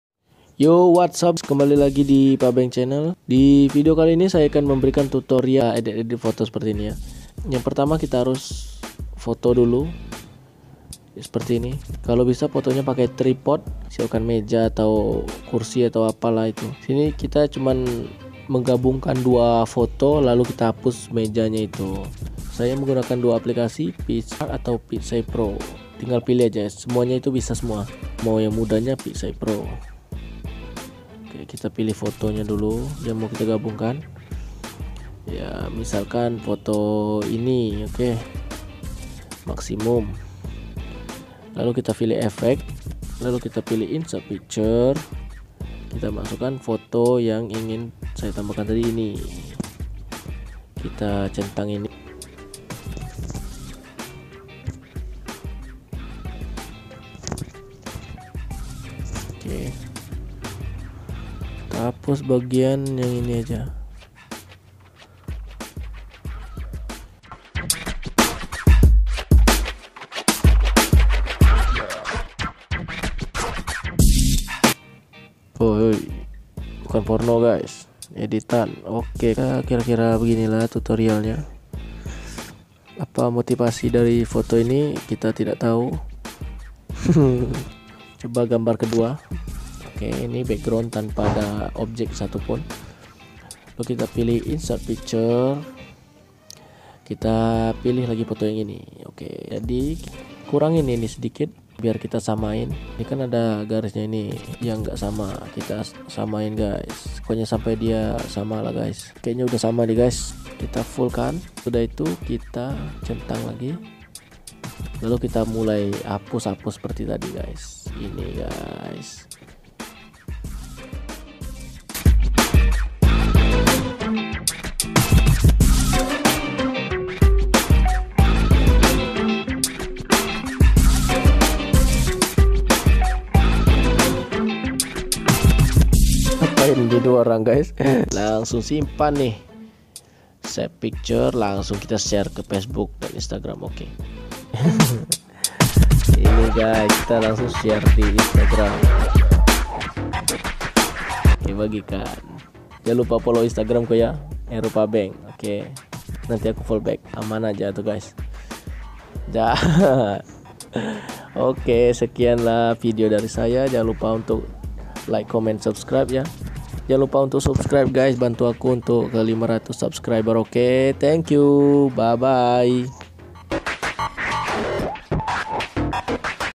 Edit foto melayang keren paling mudah (cocok untuk di tarok sosmet)
Di video ini saya sedikit memberikan tutor mengedit foto paling simple..
Jgn lupa
Bantu saya ke 500 sub..
Thanks teman-teman
Sosmet
Heru kurniawan pabeng
Heru_pabeng
Baiiii baiiii baiiiii